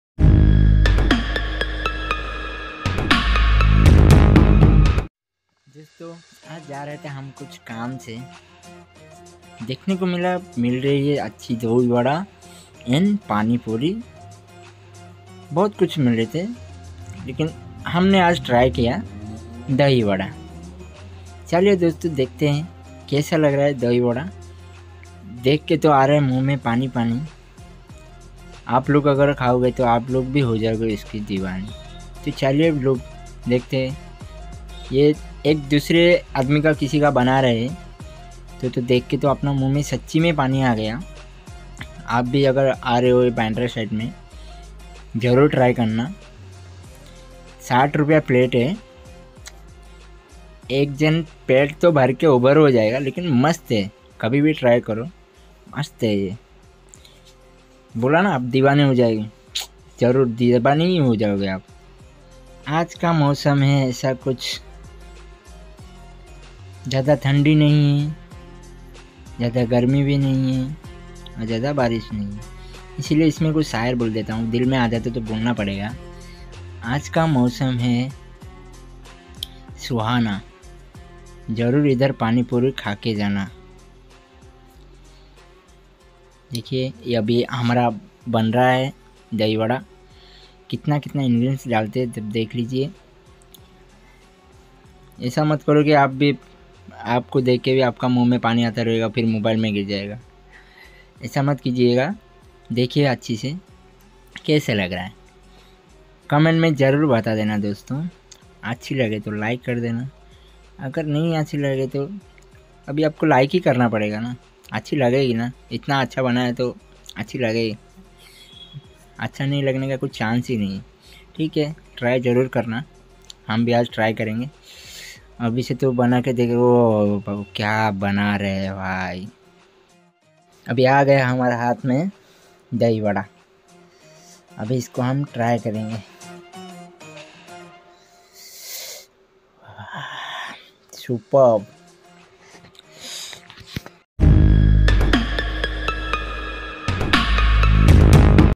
Street Style Dahi Bhalla Recipe | Street Food
Dahi Bhalla Is one Type Of  chaat Organization From The Indian Cuisine| Dahi Bhalle Is A Sumptuous Dish, Made with Curd,Spices&veggies | Here's a Different Way Of Preparing This Yummy & Soft Dahi Bhalla .